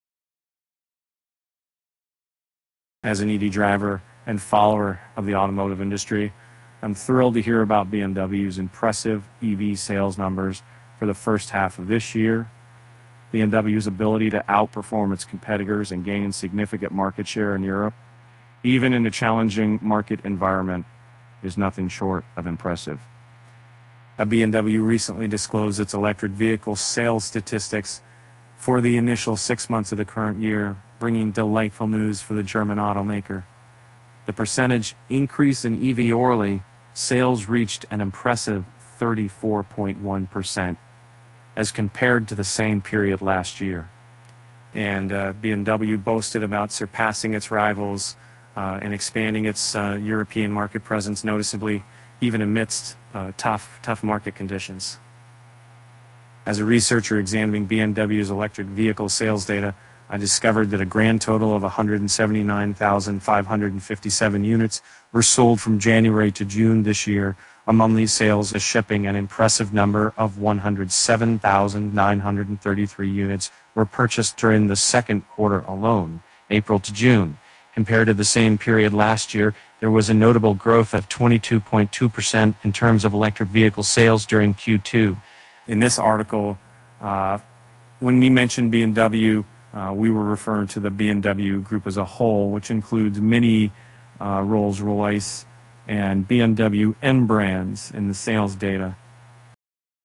BMW's EV sales jump 34% in the first half of the year
As an EV driver and follower of the automotive industry, I'm thrilled to hear about BMW's impressive EV sales numbers for the first half of this year. BMW's ability to outperform its competitors and gain significant market share in Europe, even in a challenging market environment, is nothing short of impressive.